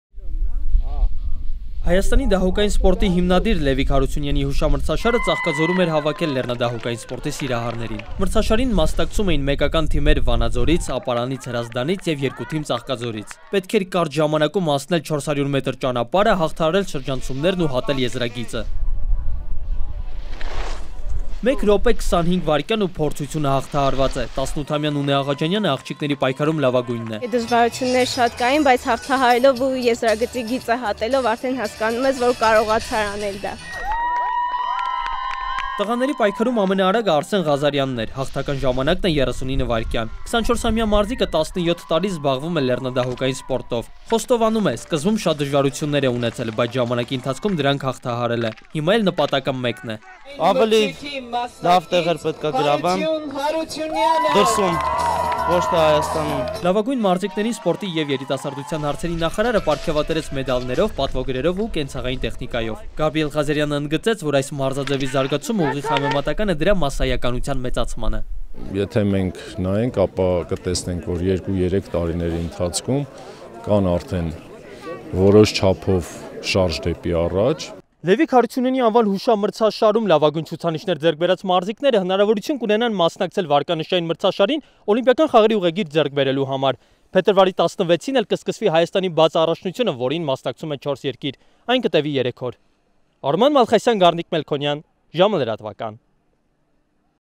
Մեկնարկում է Հայաստանի լեռնադահուկային սպորտի բաց առաջնությունը
Լավագույն ցուցանիշներով մարզիկները հնարավորություն կունենան մասնակցել վարկանշային մրցաշարին: